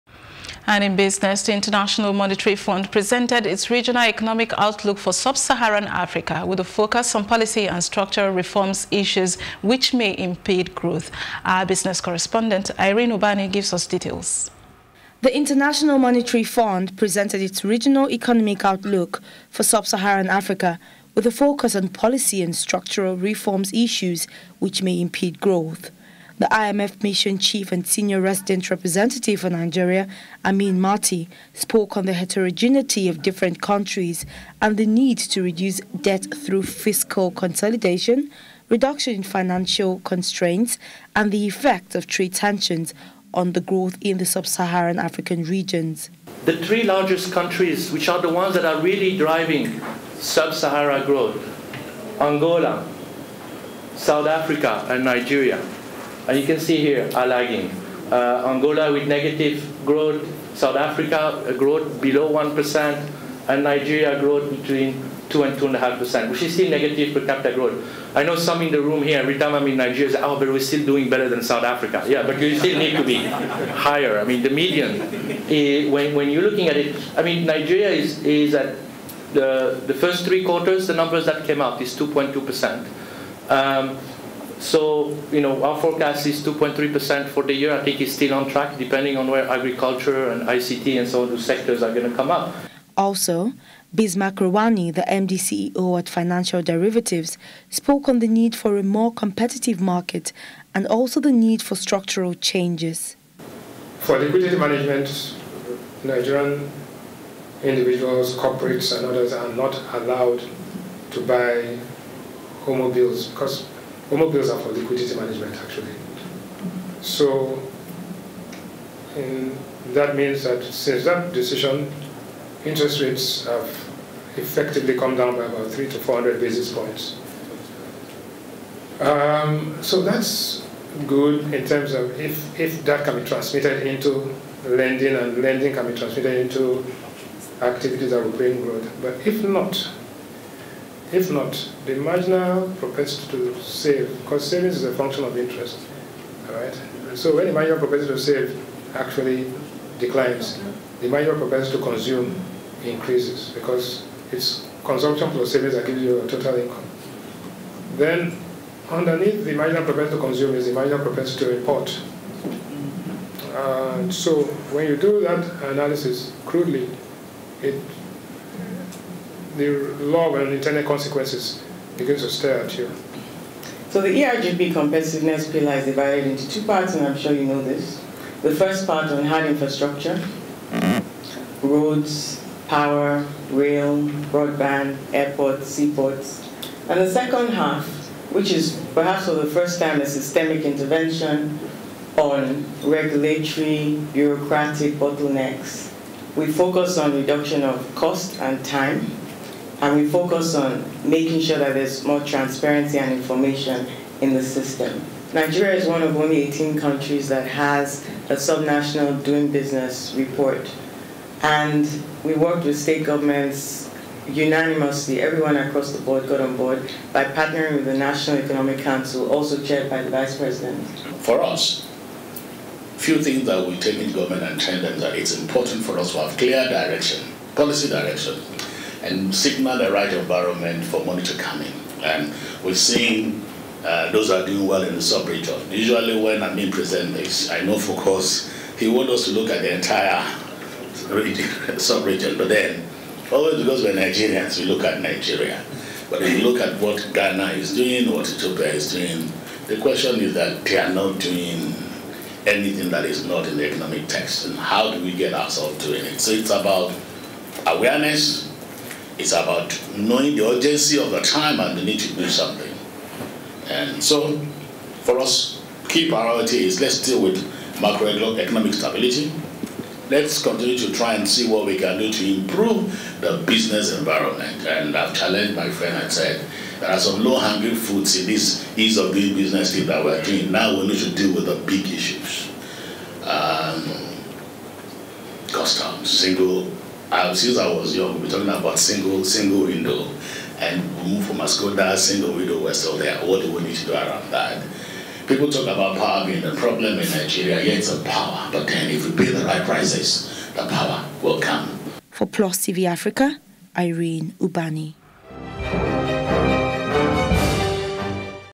IMF Regional Economic Outlook For Sub-Saharan Africa
The international monetary fund presented it's regional economic outlook for sub-saharan Africa, with a focus on policy and structural reforms issues which may impede growth.